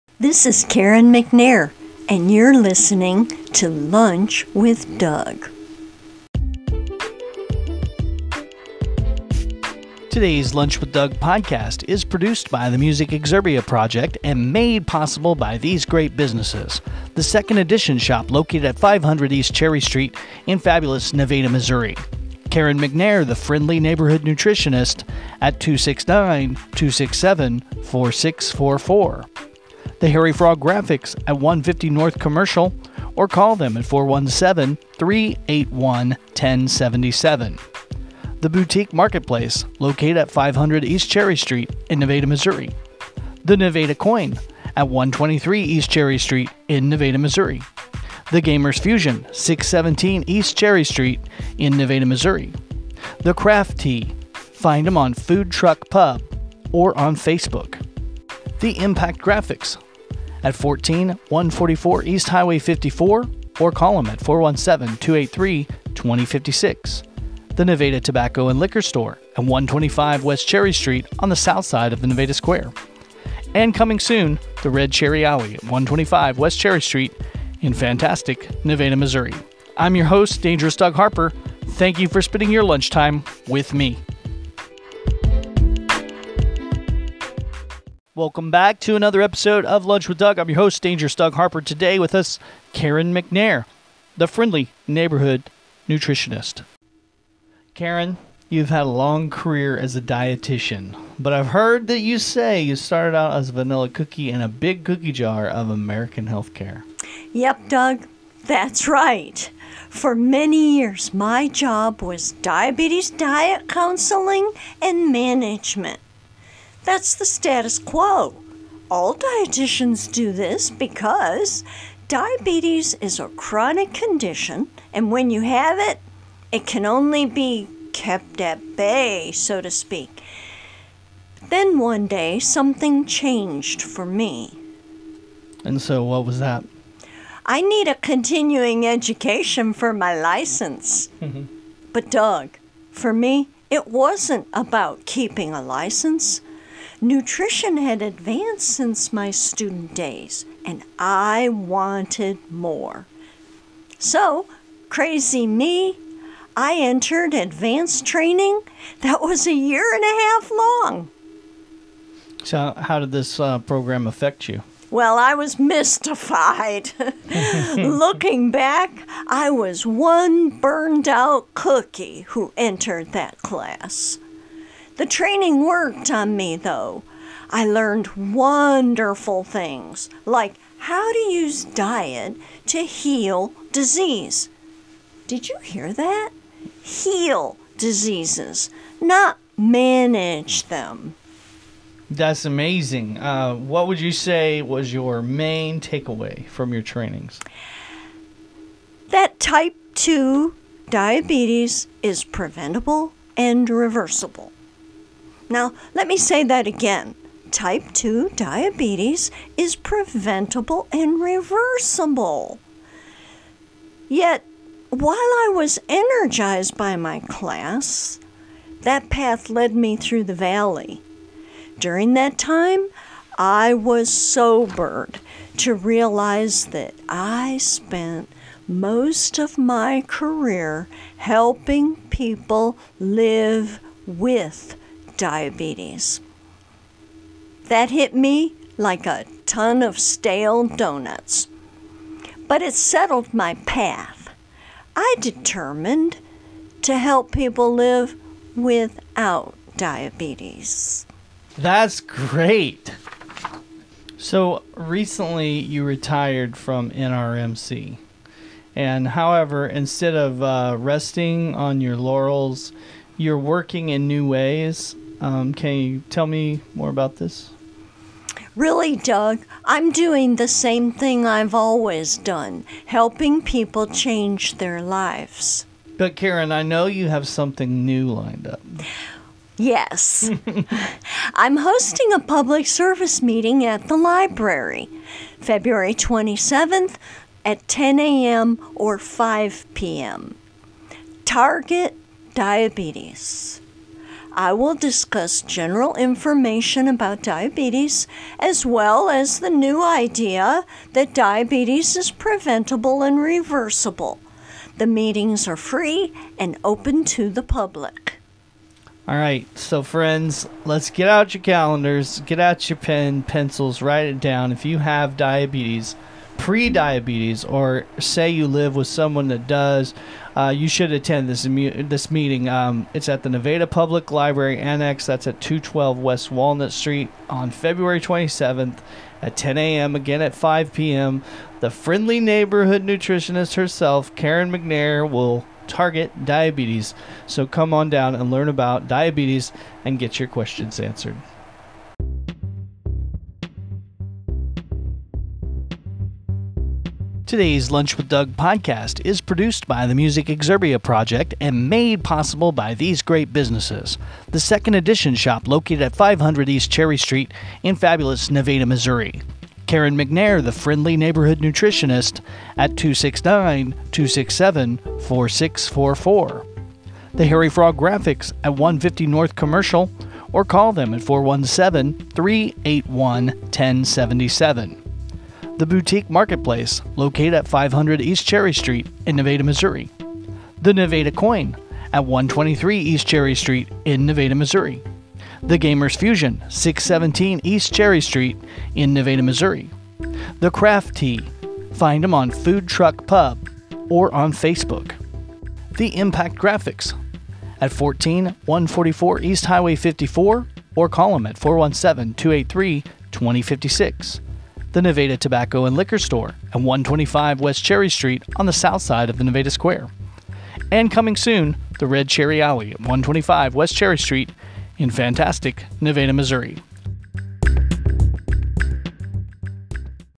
Karen McNair: Reverse Diabetes
This episode (S8:E24) we talk to Karen McNair about her program to reverse diabetes.